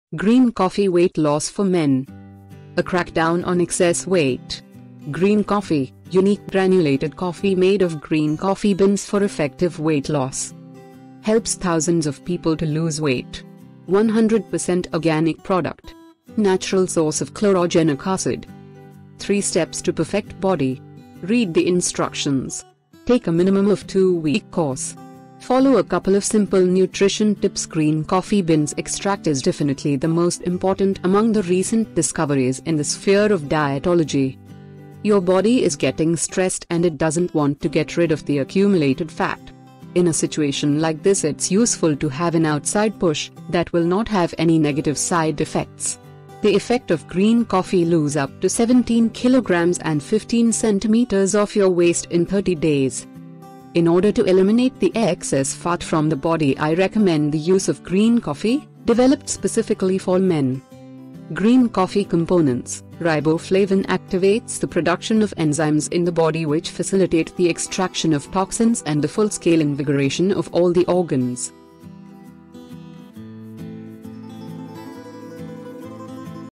Green Coffee Nescafe
GREEN COFFEE WEIGHT LOSS FOR MEN
Green Coffee is instant granulated coffee made of natural unroasted coffee beans without chemicals. Losing weight by exercising and diet restrictions has it drawbacks.
Due to this, each cup of Green Coffee contains as many essential nutrients as possible. WORKING PRINCIPLE OF GREEN COFFEE: Its ingredients combine their power to fight fat from every possible angle, starting different processes simultaneously.

The combination of caffeine and chlorogenic acid makes it the most powerful natural fat burner. The product has successfully passed a wide range of international tests, which testified its safety and efficiency.
Caffeine stimulates metabolic processes while chlorogenic acid saturates cells with oxygen and eliminates fatty deposits.
The grains contain caffeine and chlorogenic acid. These two components together break down fats and fight free radicals.
The absence of harmful elements in the body leads to the production of additional energy. This in turn helps to reduce stress, and to ensure good mood and well-being throughout the day.

GREEN COFFEE COMPONENTS:
TAURINE Straightens out digestion. Accelerates the extraction of cholesterol from the body, suppressing its absorption in the intestine.
GUARANA Uses your own fat deposits as an energy source. Resulted in reduced appetite and accelerated metabolism.
CAFFEINE Fat cells break down for the production of additional energy. Toxins, as well as excess liquid are extracted from the body.
CYANOCOBALAMIN Strengthens synthesis and ability to store protein in the body. Muscles not only do not leave with the fat, but also grow steadily.
SUCCINIC ACID Normalizes the functioning of the liver and kidneys. The skin tightens up. GUARANA Uses your own fat deposits as an energy source. Resulted in reduced appetite and accelerated metabolism.
RIBOFLAVIN Activates the production of enzymes in the body which facilitate the extraction of toxins and the full-scale invigoration of all the organs.